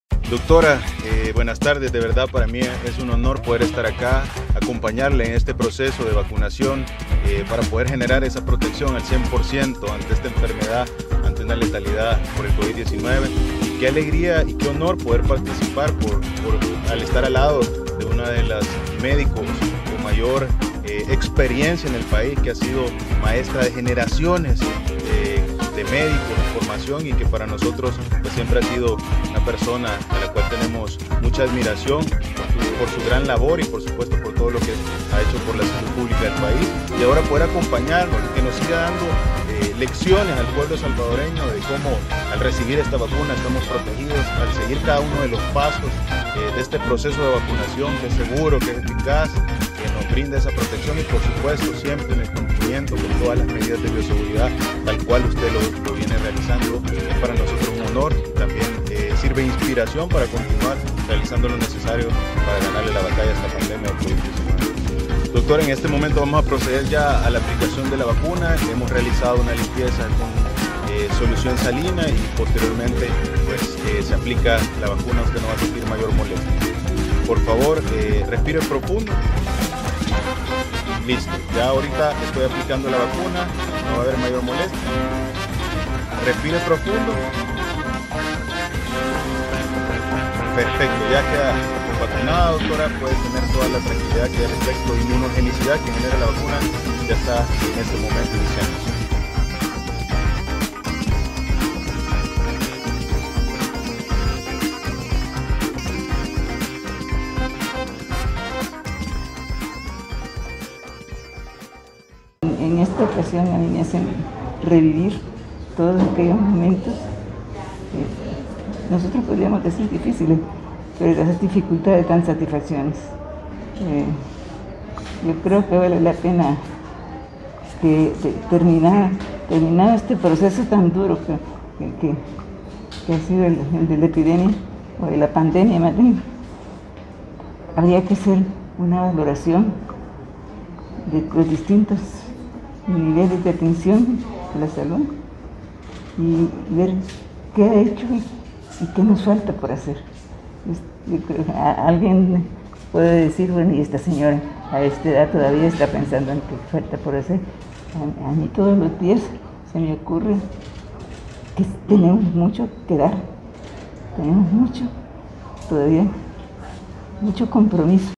Alabi con HUMILDAD VACUNA ex MINISTRA de Salud
Con una gran trayectoria, dedicándose a lo largo de su vida a su compromiso con la ciencia y la educación, logrando el reconocimiento como Heroína de la Salud Pública de las Américas.

Hoy, la Dra. Isabel Rodríguez ha iniciado su proceso de inmunización contra el COVID19.